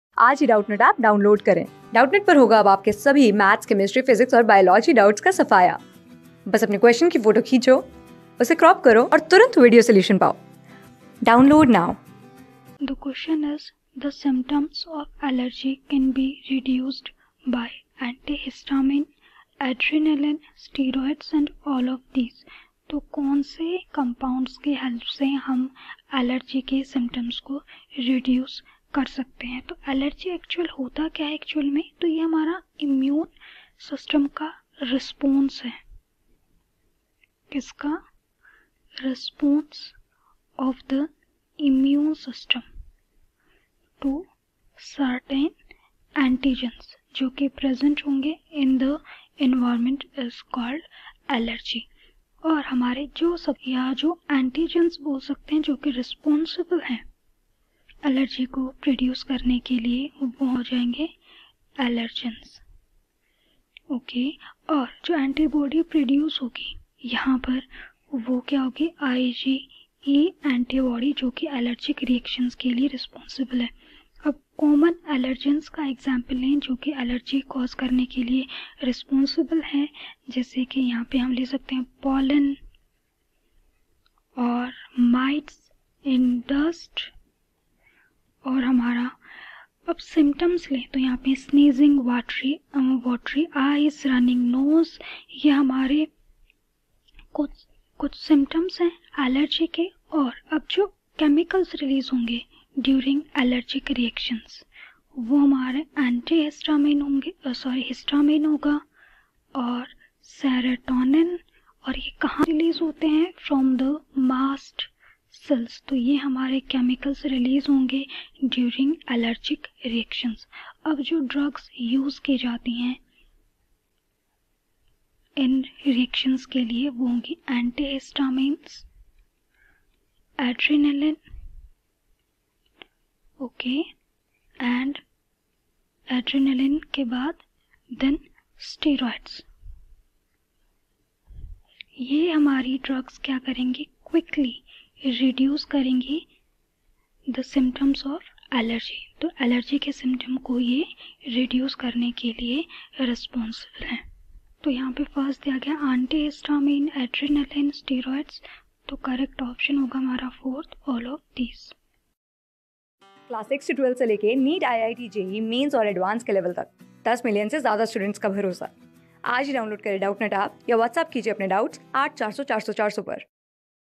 The symptoms of allergy can be reduced by | CLASS 12 | NEET MOCK TEST 12 | BIOLOGY | Doubtnut
The symptoms of allergy can be reduced by
Class: 12
Subject: BIOLOGY
Chapter: NEET MOCK TEST 12
Board:NEET

👉 Have Any Query? Ask Us.